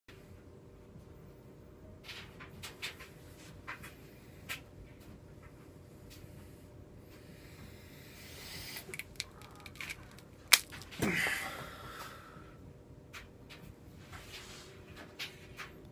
Mashed Potato
Making mashed potatoes by the hands of Hairculese.